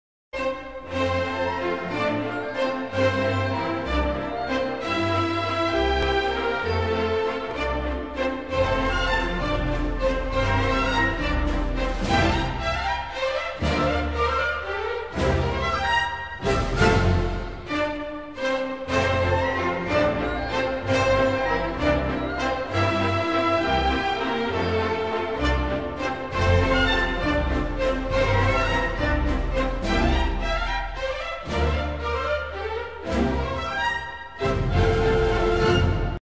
Time Lapse Photography of Flowers Blooming - Hello Spring!
As winter gives way to spring, various flowers are blooming. Using time lapse photography, let's listen to the voice of flowers, and enjoy the blooming of life.